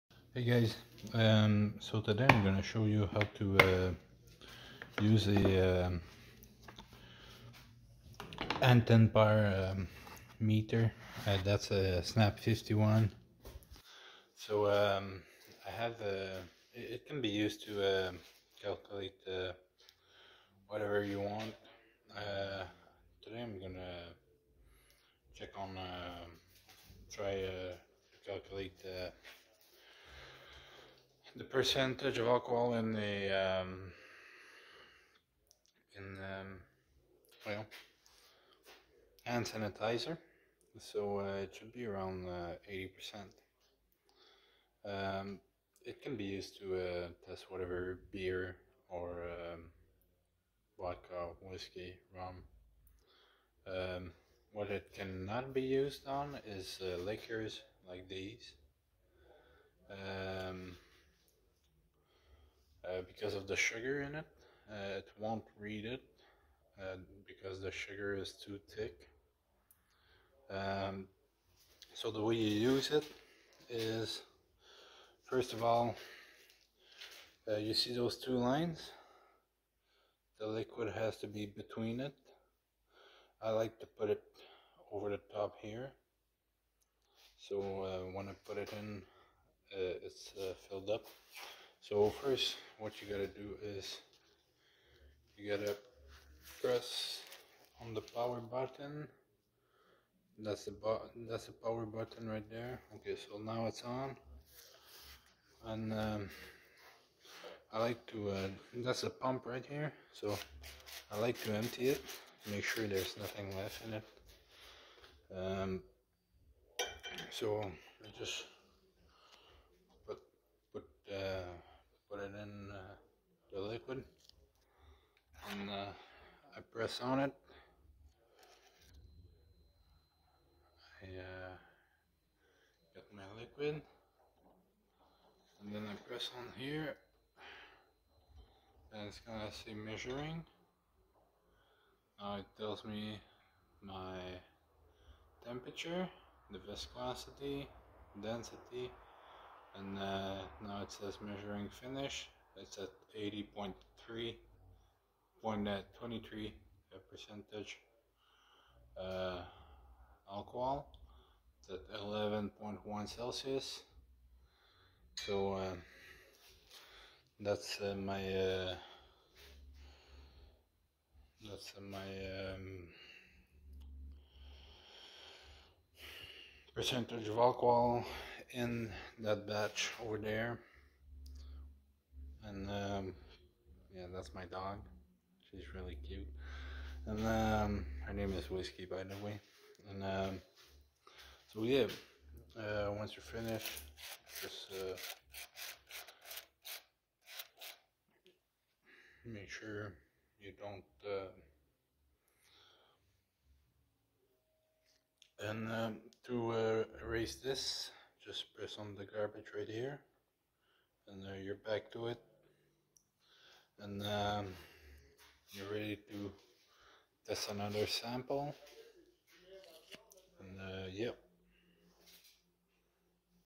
Anton paar snap 51 how to use
How to use anton paar snap 51 to mesure alcohol content in : hand sanitizer, beer, vodka, rum, whisky, wine, windshield washer, defroster...